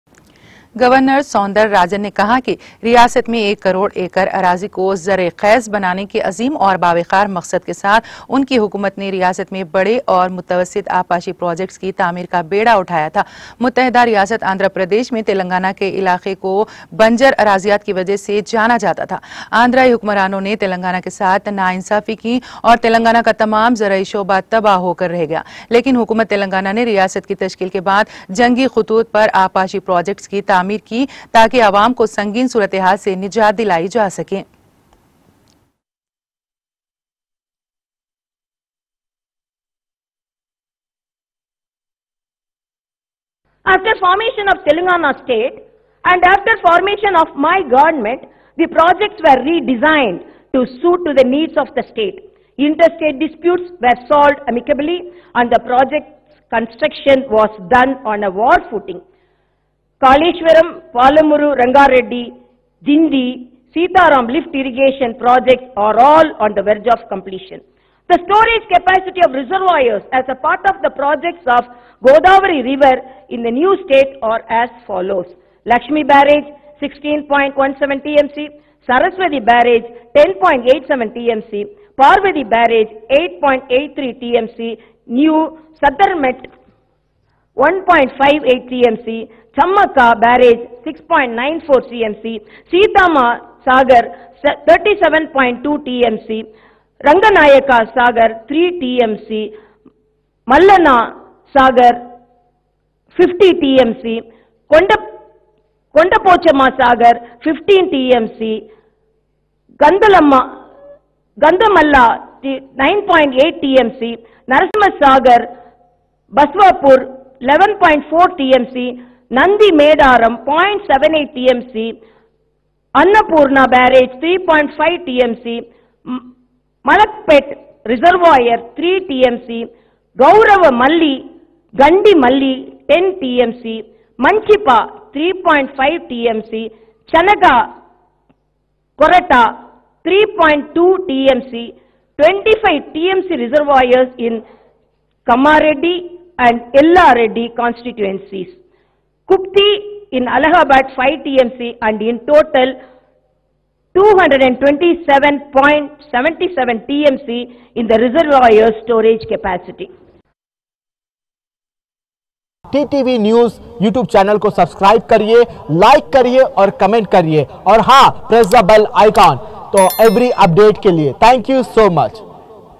TS GOVERNOR TAMILISAI SUNDERRAJAN NEY ASSEMBLY MEY IRRGATION PROJECTS PER KHITAB KIA T TV NEWS
TS GOVERNOR TAMILISAI SUNDERRAJAN NEY ASSEMBLY MEY IRRGATION PROJECTS PER KHITAB KIA T TV NEWS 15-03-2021